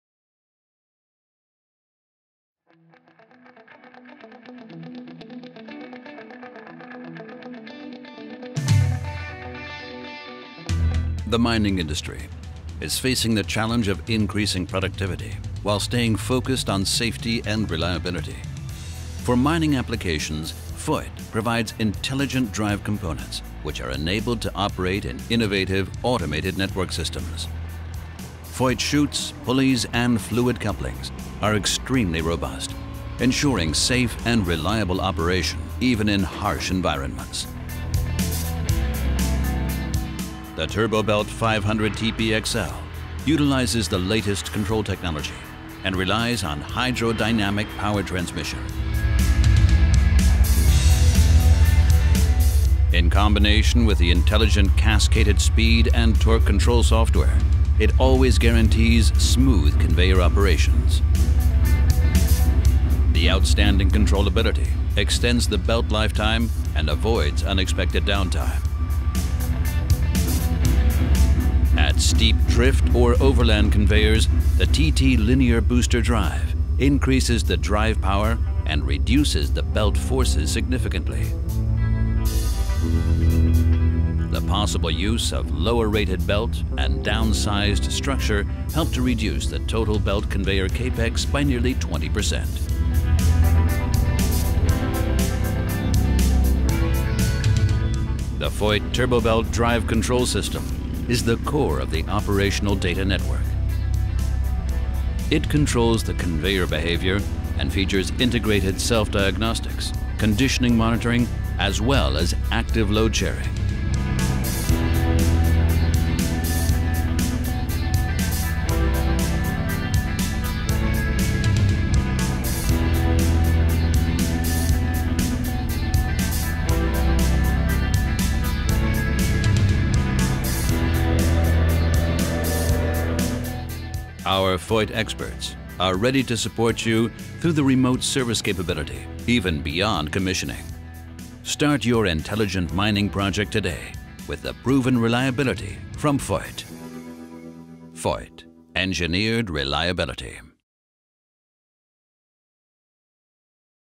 Voith Animation Intelligent Mining (EN)
Animation zu den Produkten im Bergbau von Voith.